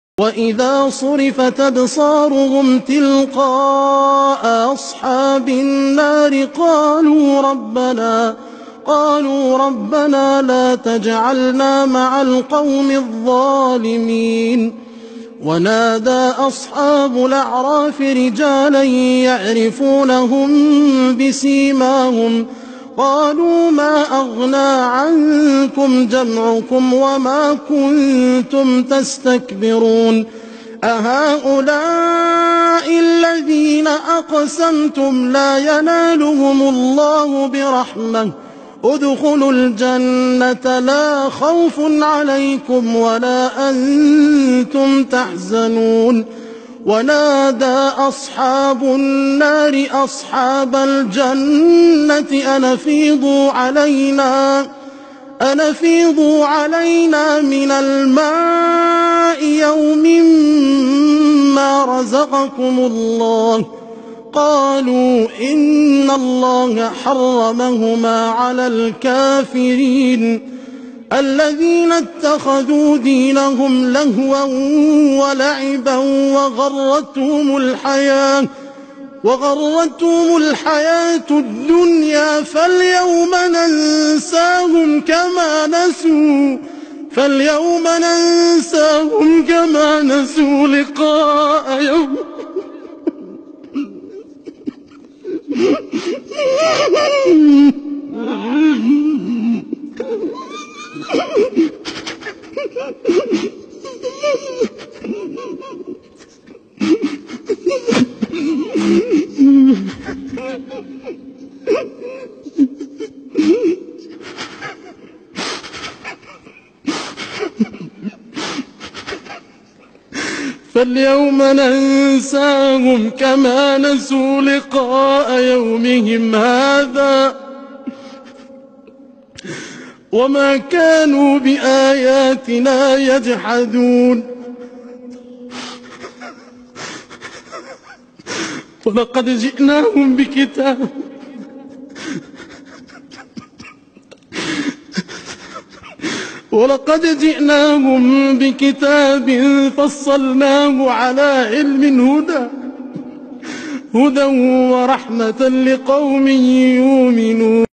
Allow u dambi dhaaf Qofkii qalbikiisa ku weyn yahy Alla ka cabsiga.aamin🤲yaa allaah. Walal Daawo w
Allow u dambi dhaaf Qofkii qalbikiisa ku weyn yahy
Alla ka cabsiga.aamin🤲yaa allaah.

Walal Daawo walaahi Qabikaga haduu qoyan yahy
Waa ilmayn.